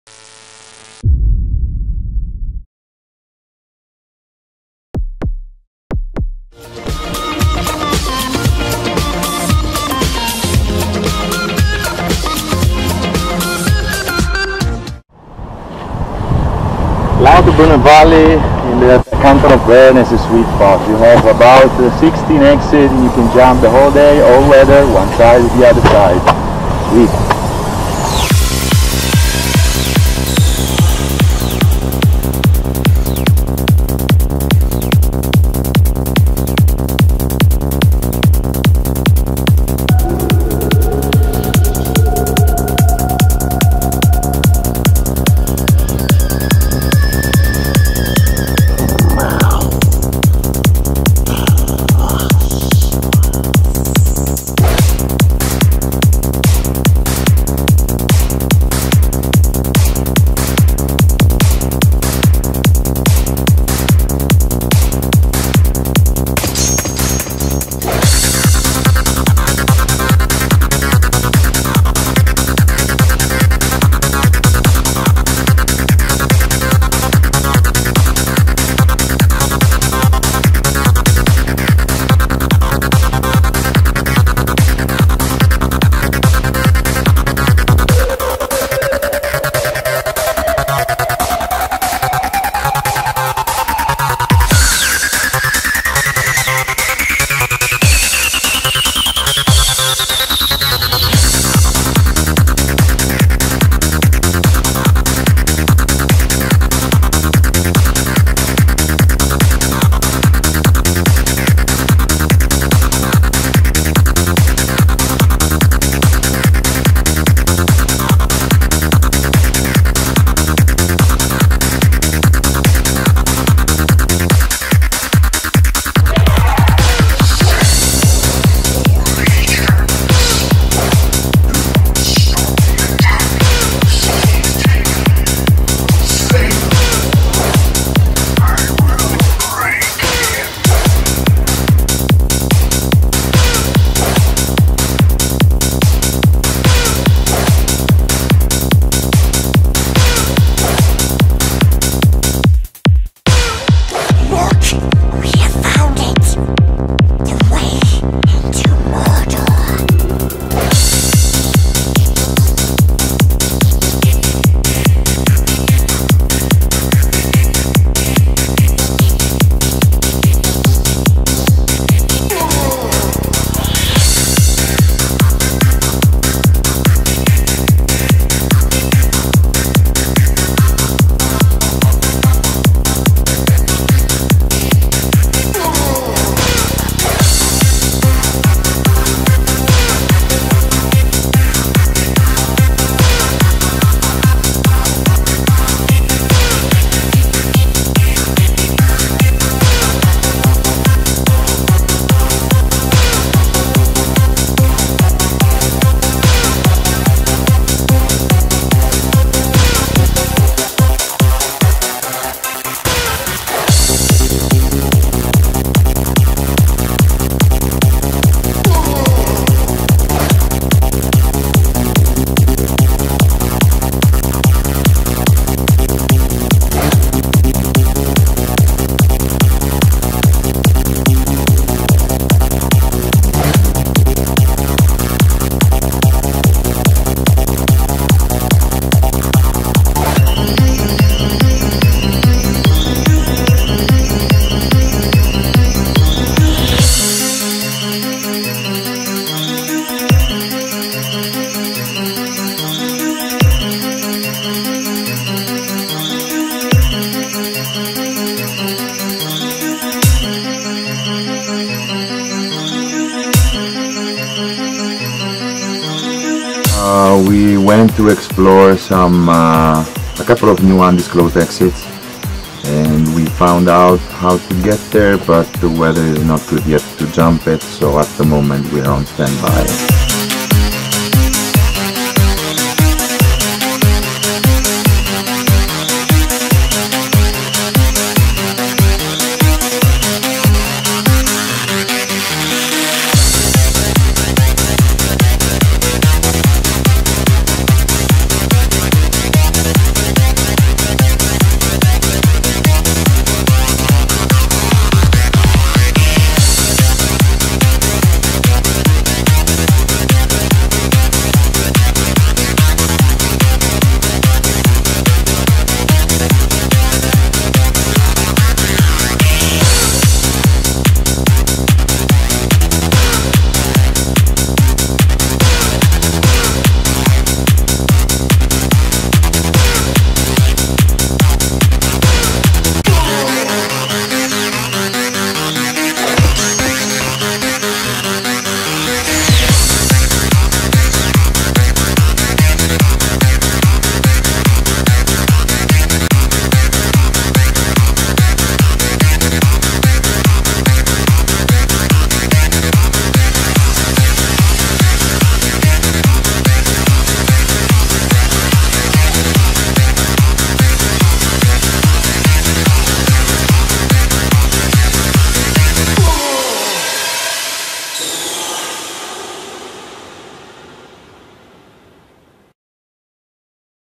Dam BASE Jumping! | Aerial Chronicles of a Venetian, Ep. 3
Oh you ragamuffin daredevils, always jumping off damn dams! Vladimir Spigler and his buddies have no qualms about BASE jumping off anything and everything -- in this video they're leaping off mountains in Lauterbrunnen, Canton of Bern and Canton of Wallis.

Vladimir and his Italian crew then move to the Western part of the Swiss Alps to discover some new undisclosed jumps, like a huge 250m concrete wall dam and a sweet jump next to a fairy waterfall...

The journey was made difficult by unlucky weather - wind rain and low clouds but patience and respect for nature is a must when trying to be successful in the unknown mountain environment...

Athletes: Vladimir Spigler, Alexander Edbom, Peter Salzmann, Olivier Chacornac, Kristoffer Andersen,Twin Pask